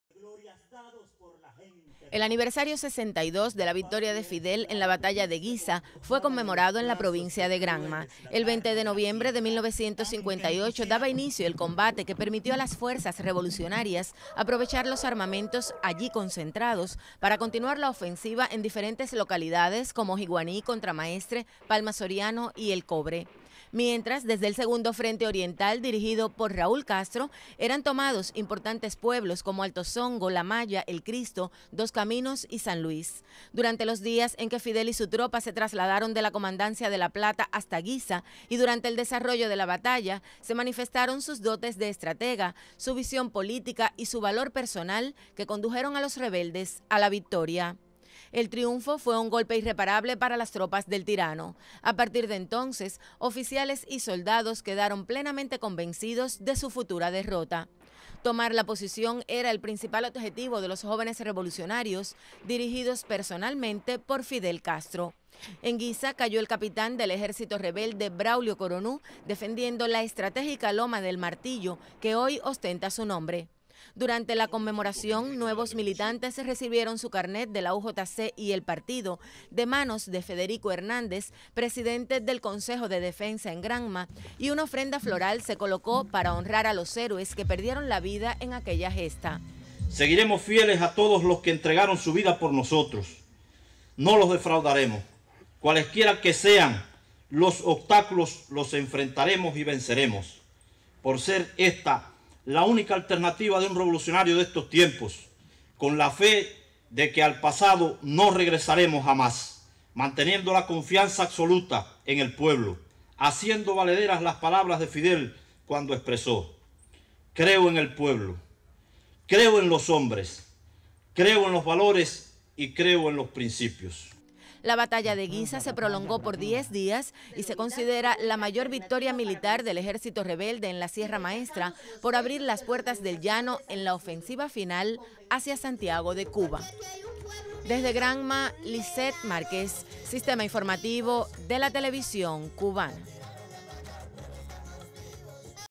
Conmemoran Aniversario 62 de la Batalla de Guisa dirigida por Fidel
El aniversario 62 de la Batalla de Guisa dirigida personalmente por el Comandante en Jefe Fidel Castro fue conmemorado en la provincia de Granma.  El hecho es considerado una de las mayores victorias del Ejército Rebelde en la Guerra de Liberación en Cuba